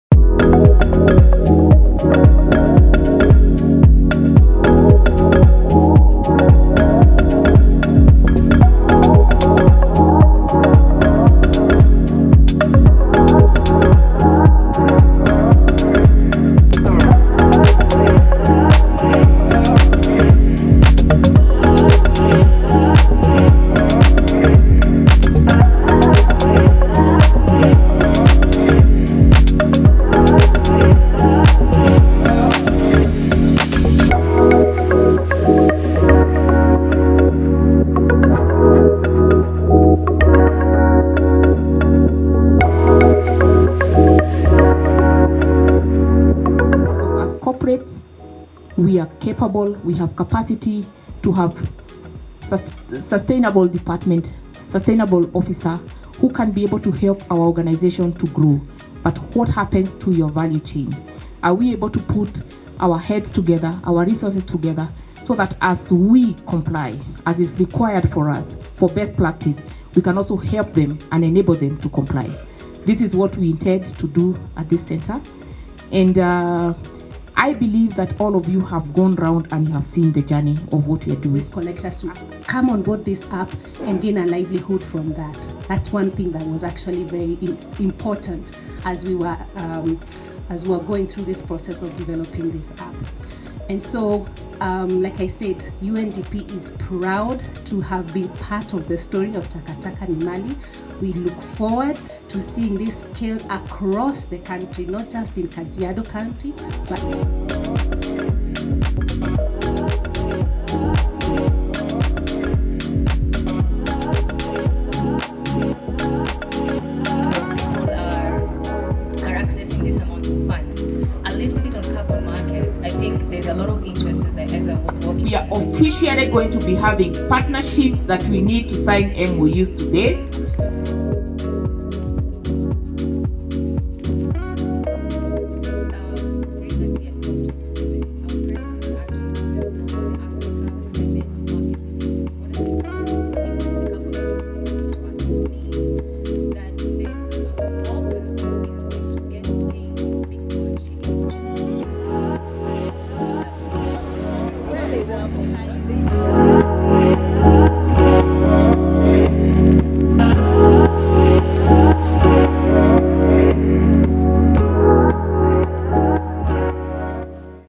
TTNM LAUNCH EVENT VIDEO
TakaTaka Ni Mali is a waste management not-for-profit organization whose mandate is to strengthen connections by linking solid waste generators, users, and recyclers in driving the agenda for solid waste management to achieve circularity.

At Crestwood, we provide Integrated Marketing Communication Services for the organization.